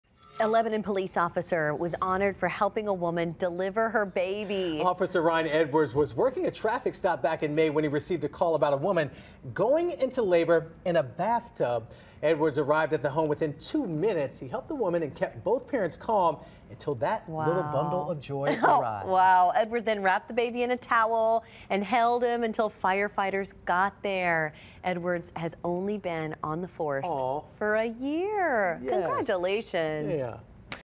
Lebanon officer helps deliver baby
Edwards was able to rush in and render aid to the mother during her delivery.